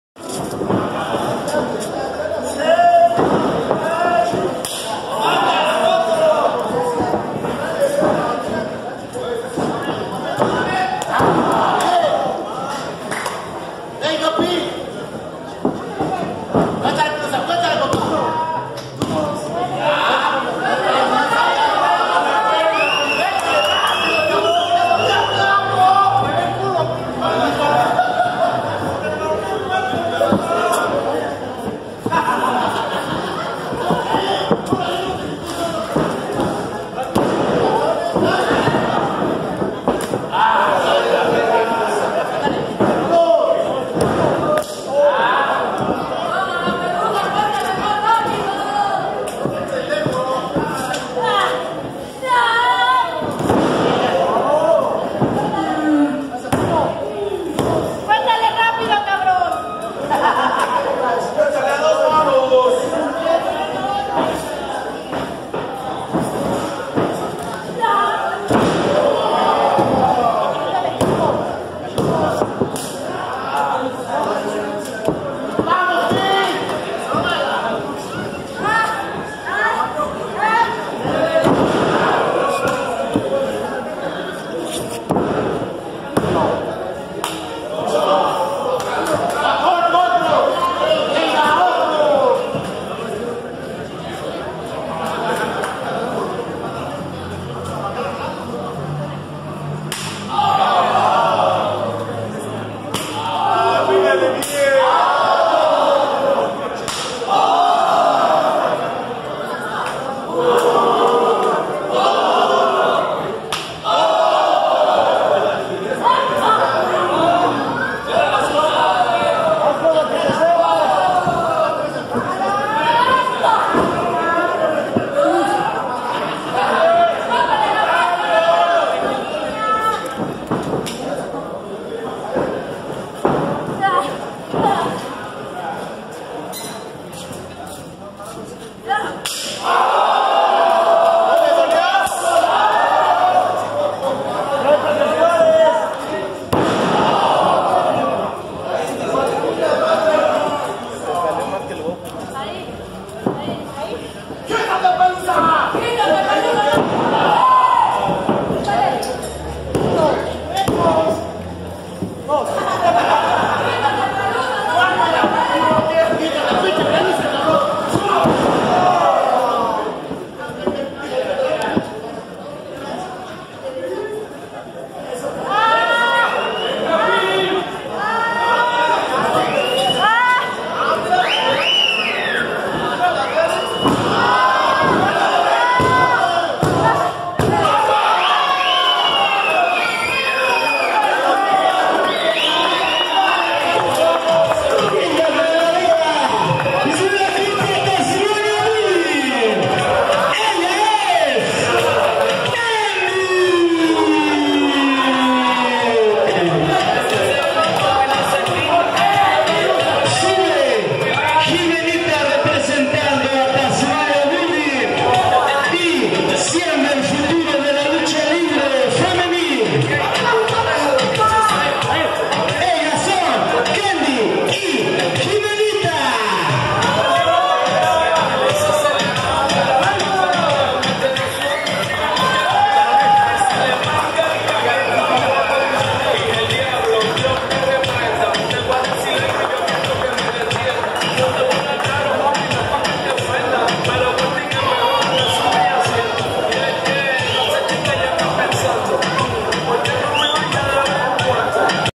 KENDY REBEL SE LLEVA EL TROFEO A CASA !!!
Después de un toma y daca constante Kendy se luce para ganar la Copa Tazmania Bully !!!